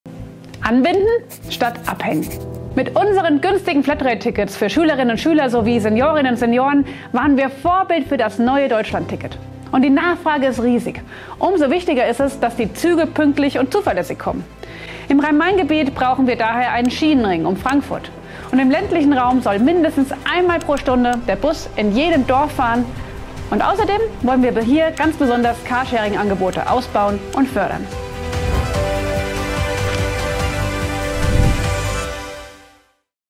11 Gründe für GRÜN: Anbinden statt abhängen.
Wir arbeiten an einem Schienenring um Frankfurt, dem Ausbau und der Förderung von Carsharing-Angeboten im ländlichen Raum und Bussen zu jeder Stunde in jedem Dorf. Deshalb bei der Landtagswahl am 8. Oktober mit beiden Stimmen GRÜN wählen! /P